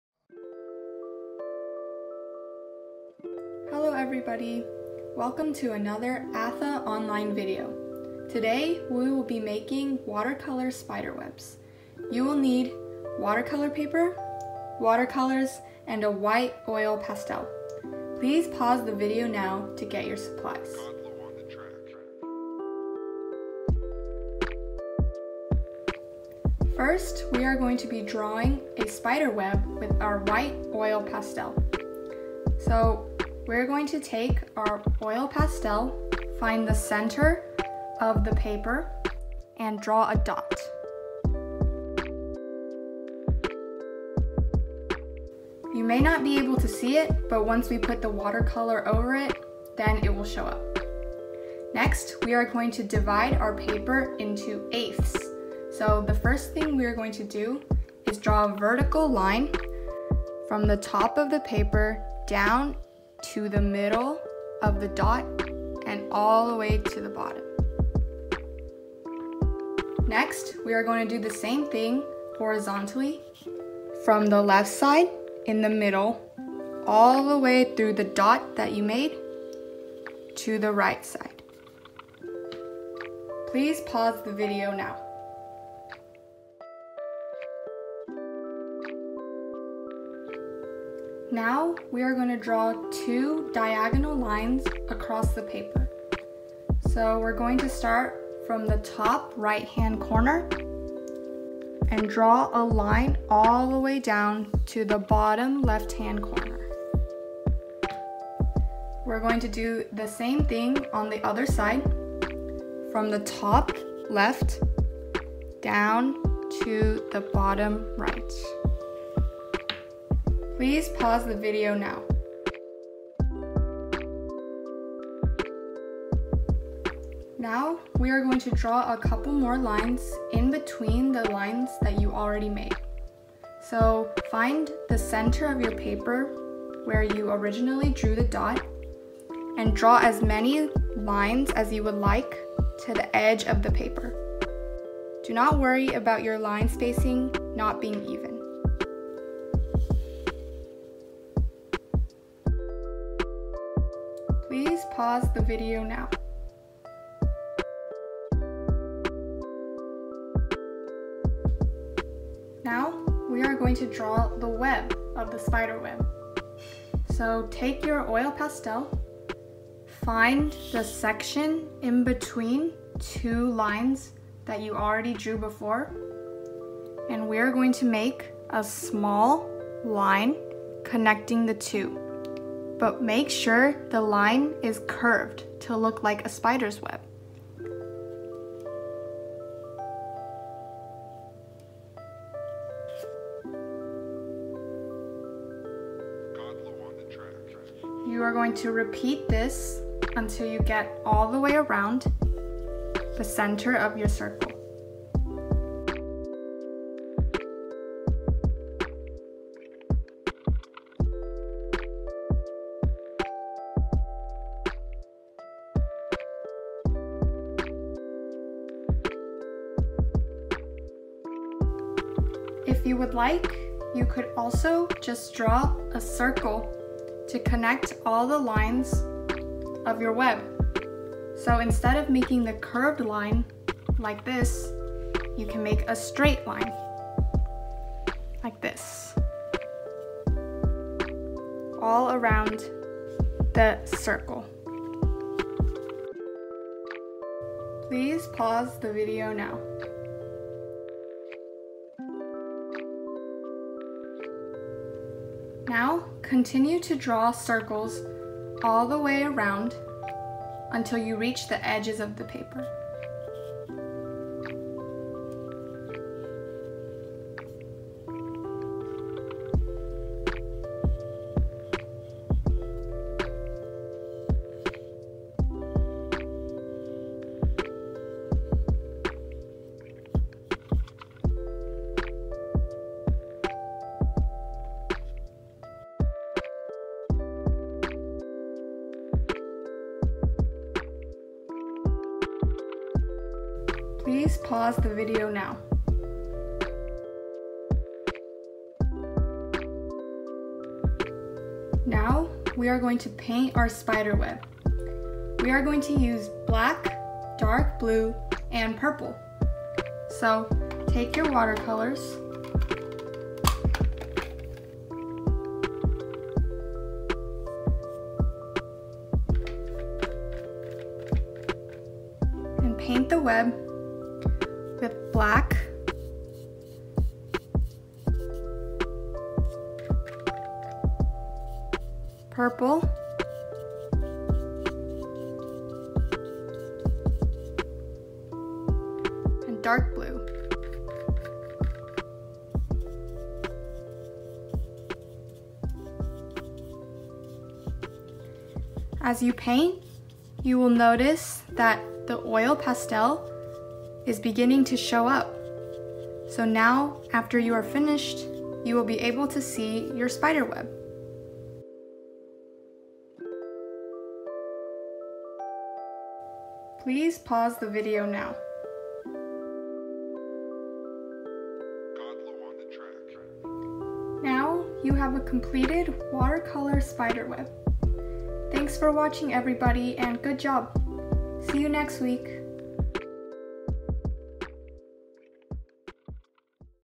ATHA Online: Spooky Spider Web
Hello everyone! Today we're making a spider web drawing using oil pastels and watercolor paints! Parents, we recommend you watch through the entire video once before helping your child begin the project. Enjoy!